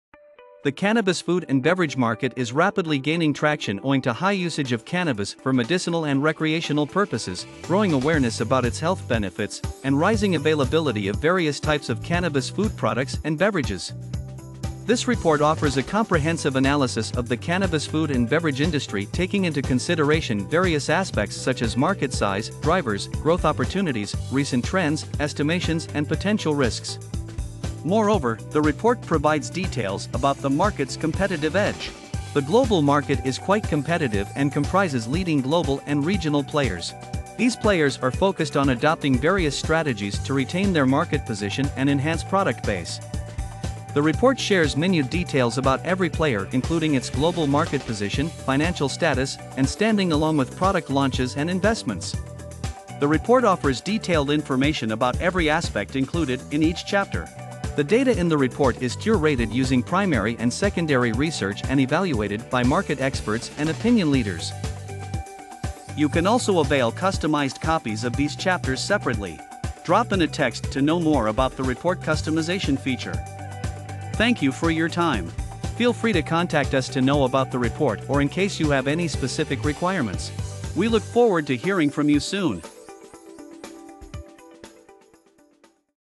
Cannabis Food and Beverage Market Set to Grow Positively
Global cannabis food and beverage market is anticipated to grow at a CAGR of 4.5 % from 2022 to 2028. This growth can be attributed to the increasing legalization of cannabis for medical and recreational purposes in various countries, as well as the rising demand for cannabis-infused products.
The report also highlights the growing trend of using cannabis in food and beverage products, including baked goods, chocolates, gummies, beverages, and others. With the increasing awareness of the potential health benefits of cannabis, such as pain relief and relaxation, more consumers are turning to cannabis-infused products as an alternative to traditional pharmaceuticals.